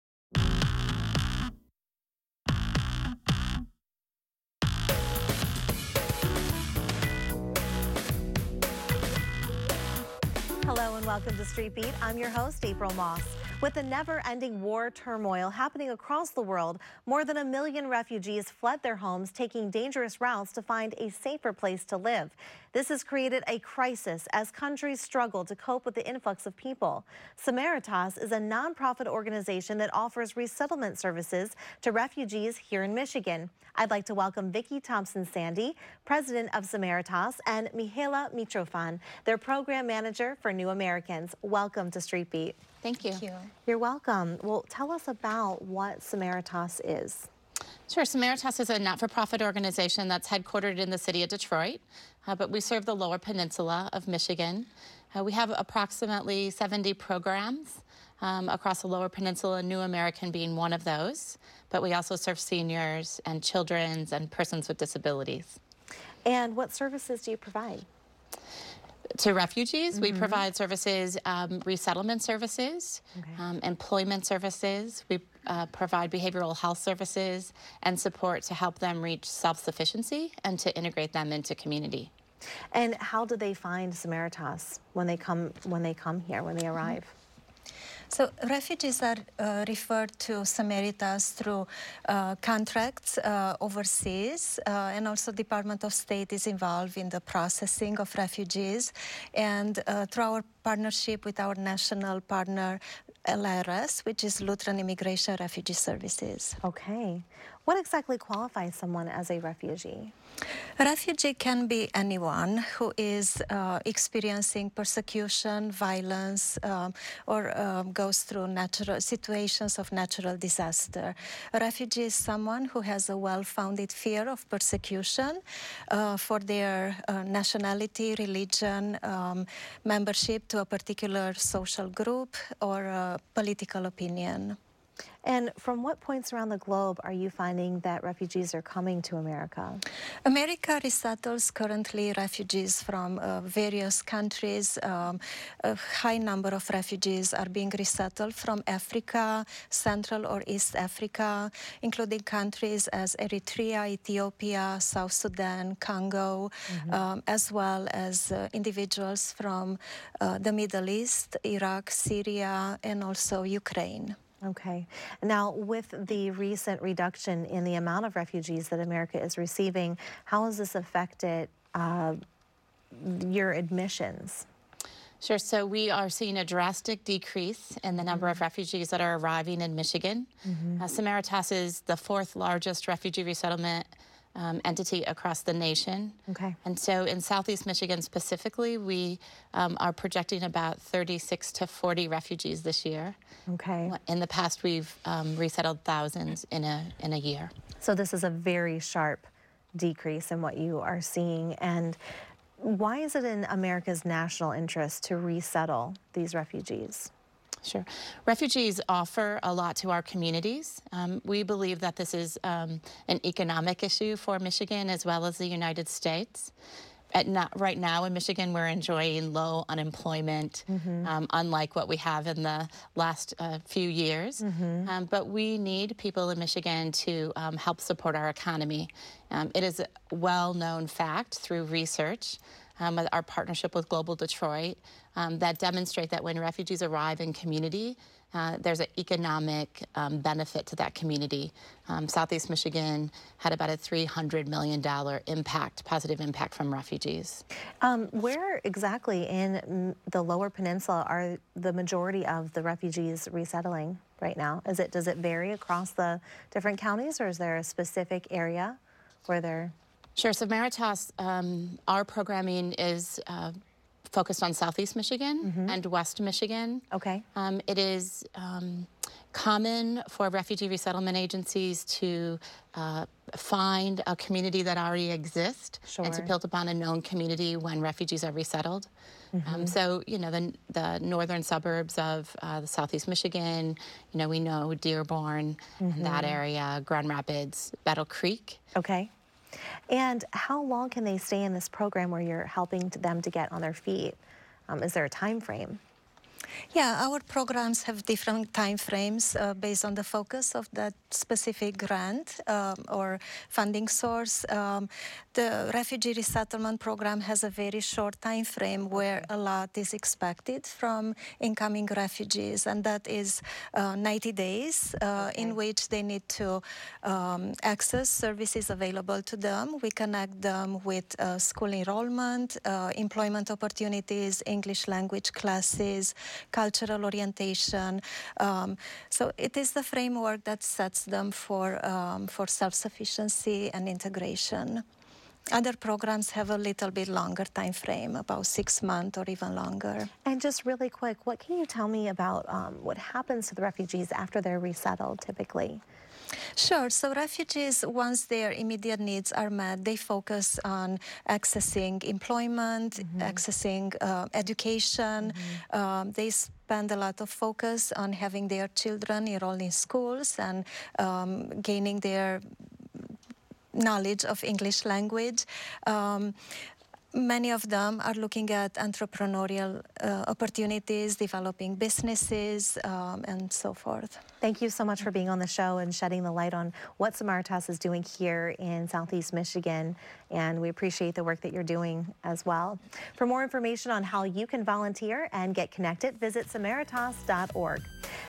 Street Beat: Detroit's Response To The Refugee Crisis « CW50 Detroit
WKBD Street Beat | 14 April 2018 | Your People secured this interview with CW50 on behalf of our client, Samaritas. Vickie Thompson-Sandy, the President of Samaritas, and the Program Manager for New Americans, Mihaela Mitrofan, discuss refugee resettlement.